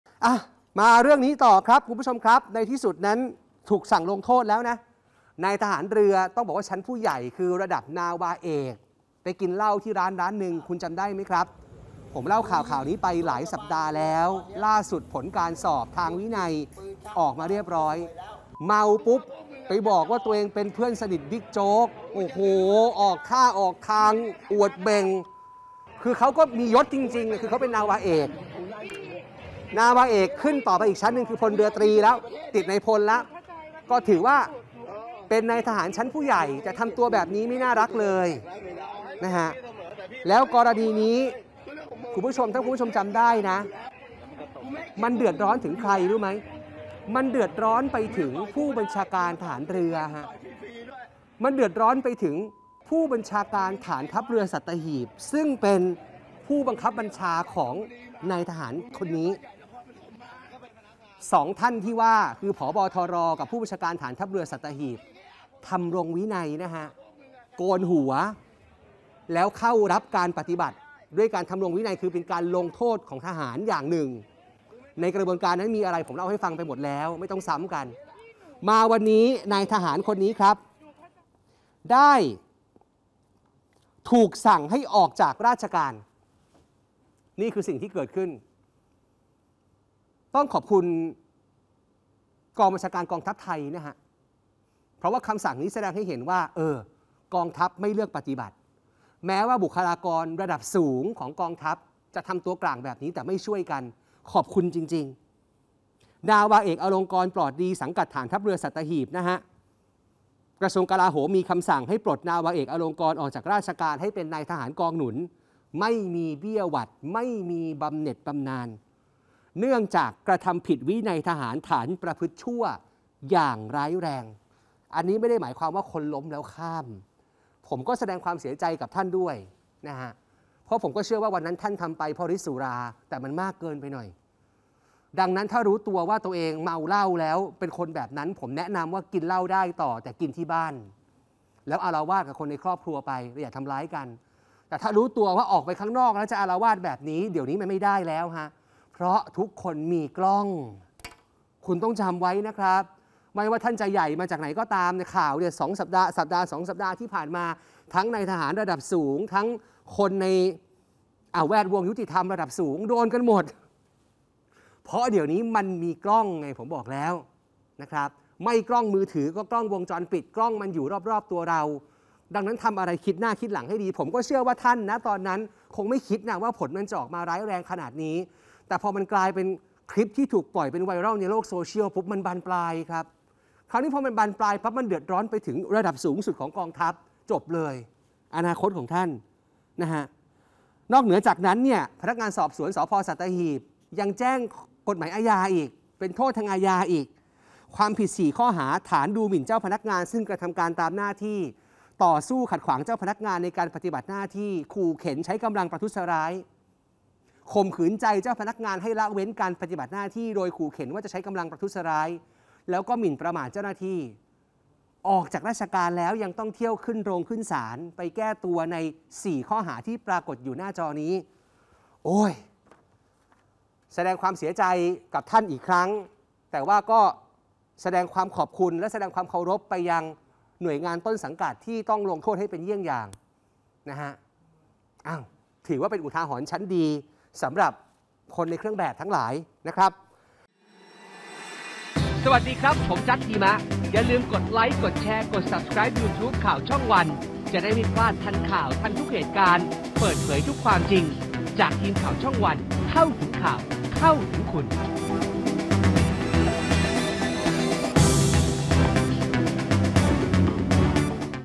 สั่งปลด "น.อ.คลิปฉาว" เมาขู่ตำรวจ | ข่าววันศุกร์ | ข่าวช่องวัน
ปลัดกระทรวงกลาโหม สั่งปลดทหารเรือเมาและอาละวาดในสถานบันเทิงออกจากราชการ ให้เป็นนายทหารกองหนุน ไม่มีเบี้ยหวัด บำเหน็จบำนาญ ฐาน ประพฤติชั่วอย่างร้ายแรง หลังถูกธำรงค์วินัย